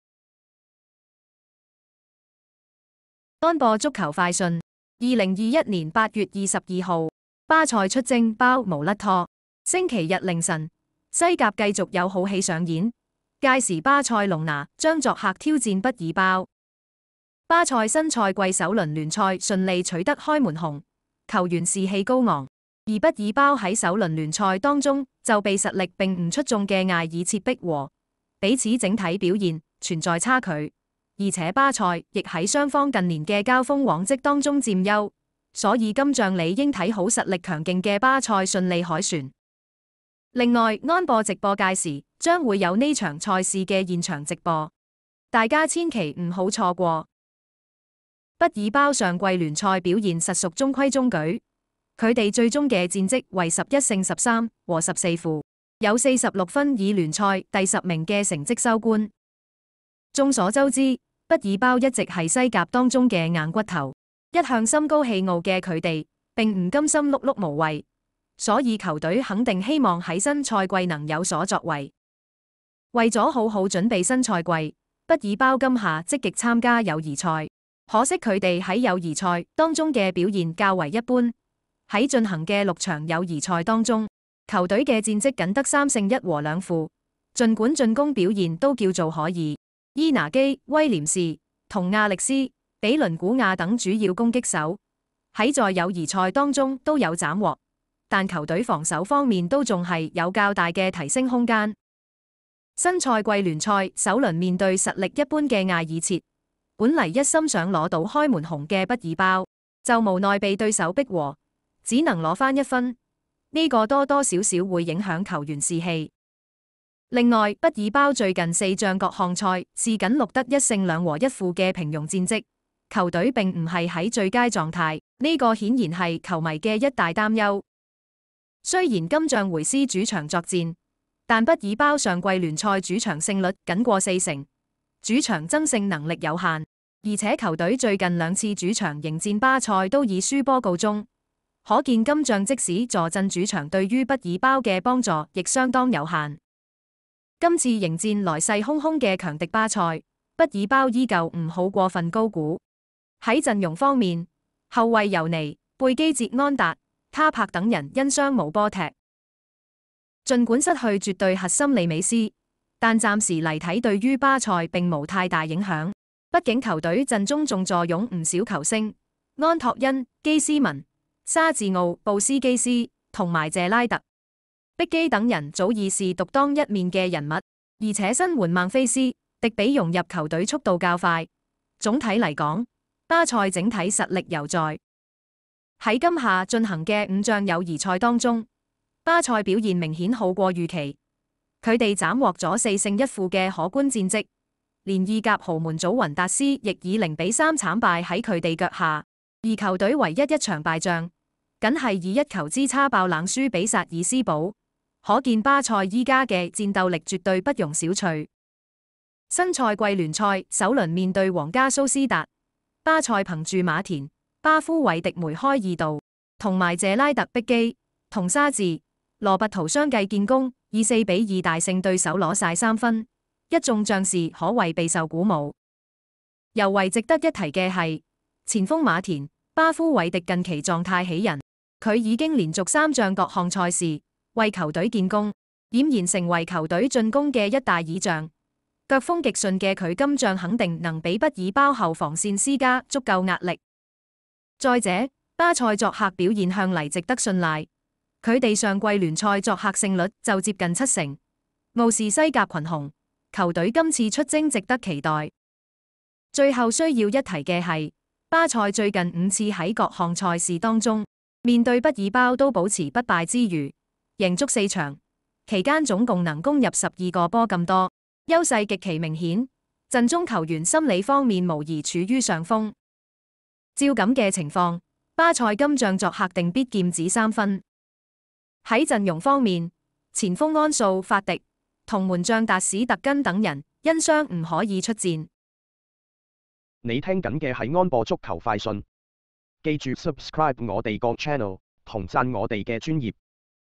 【西甲🇪🇸-賽前新聞】2021-08-22 巴塞隆拿 VS 畢爾包 | 巴塞出征「包」無甩拖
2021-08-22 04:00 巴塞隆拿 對
►立即下載M直播觀看 西甲 精彩賽事

週日凌晨，西甲繼續有好戲上演，屆時巴塞隆拿將作客挑戰畢爾包。巴塞新賽季首輪聯賽順利取得開門紅，球員士氣高昂，而畢爾包在首輪聯賽當中則被實力並不出眾的艾爾切迫和，彼此整體表現存在差距，而且巴塞亦在雙方近年的交鋒往績當中佔優，因此今仗理應看好實力強勁的巴塞順利凱旋。另外，M直播屆時將會有本場賽事的現場直播。

畢爾包上季聯賽表現實屬中規中矩，他們最終的戰績為11勝13和14負，積46分以聯賽第十名的成績收官。眾所周知，畢爾包一直是西甲當中的硬骨頭，一向心高氣傲的他們並不甘心碌碌無為，因此球隊肯定希望在新賽季能有所作為。為了好好準備新賽季，畢爾包今夏積極參加友誼賽，可惜他們在友誼賽當中的表現較為一般，在進行的6場友誼賽當中，球隊的戰績僅得3勝1和2負，儘管進攻端表現尚可(伊拿基.威廉士和阿歷斯.比倫古亞等主要攻擊手在友誼賽當中均有斬獲)，但球隊防守端方面還是有較大的提升空間。新賽季聯賽首輪面對實力一般的艾爾切，本來一心想取得開門紅的畢爾包卻無奈被對手迫和，只能取得1個積分，這多多少少會影響球員士氣。另外，畢爾包最近4仗各項賽事僅錄得1勝2和1負的平庸戰績，球隊並不在最佳狀態，這顯然是球迷的一大擔憂。雖然今仗回師主場作戰，但畢爾包上季聯賽主場胜率僅過四成，主場爭勝能力有限，而且球隊最近2次主場迎戰巴塞均以失利告終，可見今仗即使坐鎮主場對於畢爾包的幫助亦相當有限。此番迎戰來勢洶洶的強敵巴塞，畢爾包依舊不宜過分高估。陣容方面，後衛尤尼.貝基捷和安達.卡柏等人因傷缺陣。

儘管失去絕對核心利安奴.美斯，但暫時來看對於巴塞並無太大影響，畢竟球隊陣中還是坐擁不少球星，安托恩.基斯文、沙治奧.布斯基斯以及謝拉特.碧基等人早已是獨當一面的人物，而且新援孟菲斯.迪比融入球隊速度較快，總體來說巴塞整體實力猶在。在今夏進行的5仗友誼賽當中，巴塞表現明顯好於預期，他們斬獲了4勝1負的可觀戰績，連意甲豪門祖云達斯亦以0比3慘敗在他們腳下，而球隊唯一一場敗仗僅是以1球之差爆冷不敵薩爾斯堡，可見巴塞如今的戰鬥力絕對不容小覷。新賽季聯賽首輪面對皇家蘇斯達，巴塞憑藉馬田.巴夫韋迪梅開二度以及謝拉特.碧基和沙治.羅拔圖相繼建功以4比2大胜對手豪取3分，一眾將士可謂備受鼓舞。尤為值得一提的是，前鋒馬田.巴夫韋迪近期狀態喜人，此子已經連續3仗各項賽事為球隊建功，儼然成為了球隊進攻端的一大倚仗，腳風極順的他今仗肯定能給畢爾包后防線施加足夠壓力。再者，巴塞作客表現向來值得信賴，他們上季聯賽作客勝率就接近七成，傲視西甲群雄，球隊此番出征值得期待。最後需要一提的是，巴塞最近5次在各項賽事當中面對畢爾包均保持不敗之馀贏足4場，期間總共能攻入12球之多，優勢極其明顯，陣中球員心理方面無疑處於上風。綜上所述，巴塞今仗作客定必劍指3分。陣容方面，前鋒安素.法迪和門將達史特根等人因傷倦勤。